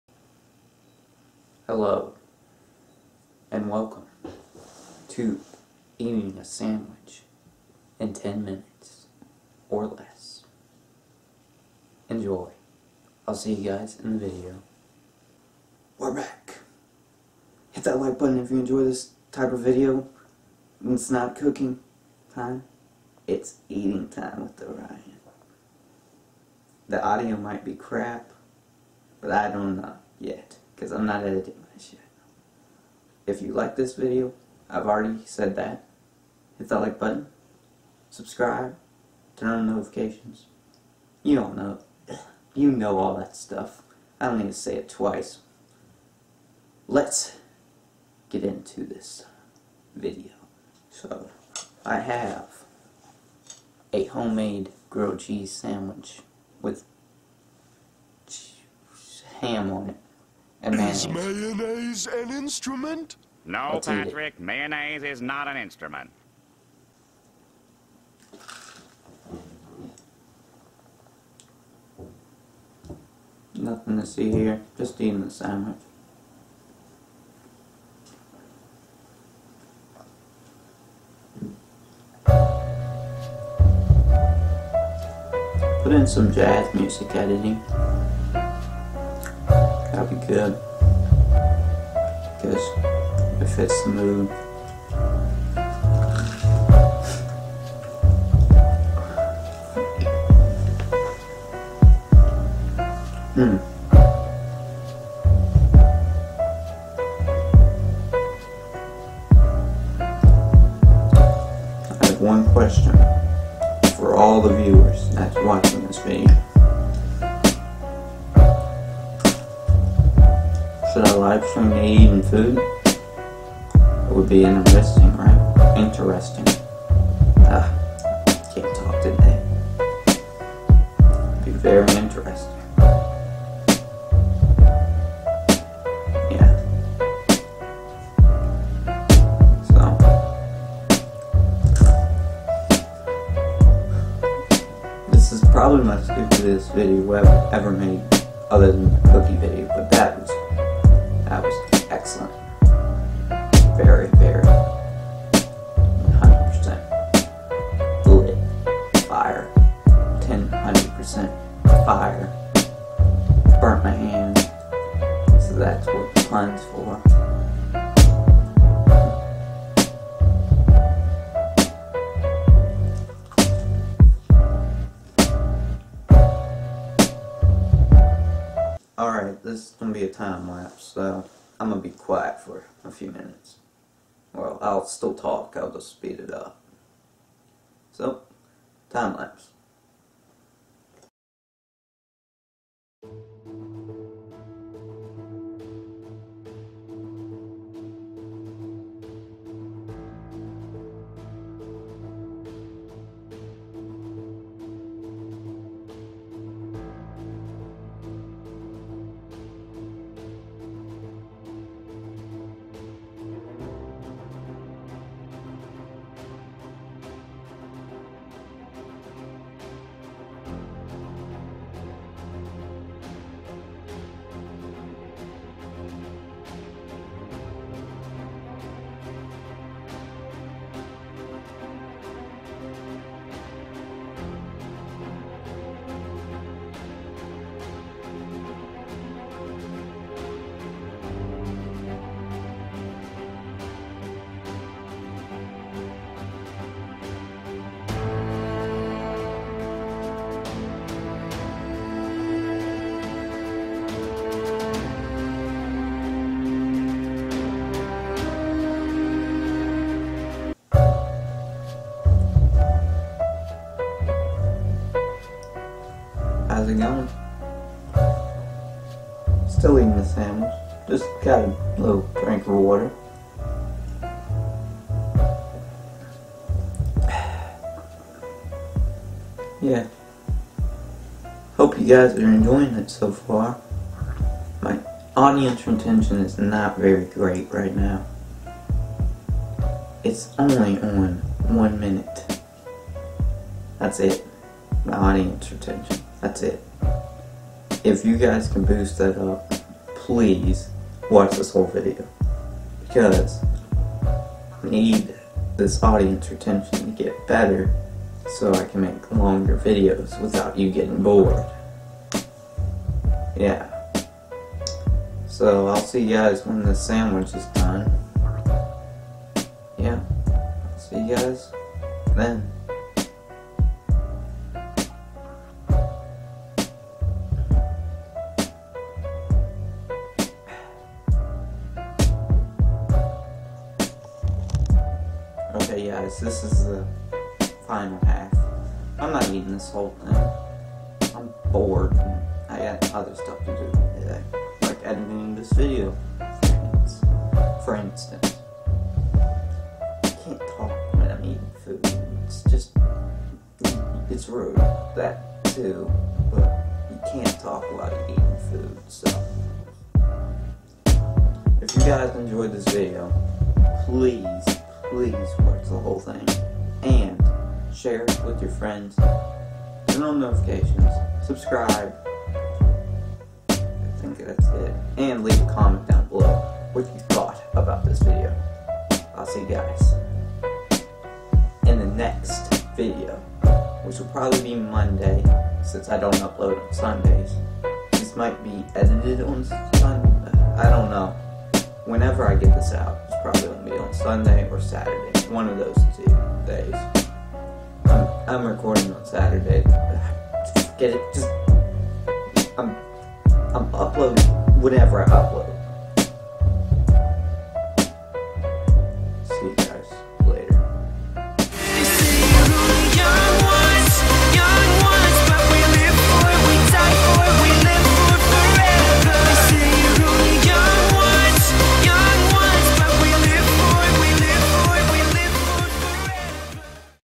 Eating A SandWich!
I like sandwichs, heres episode one of eating with orion.

Official Subreddit!

Hope you Enjoyed! And As Always Stay Super Chill my friends!

~ Orion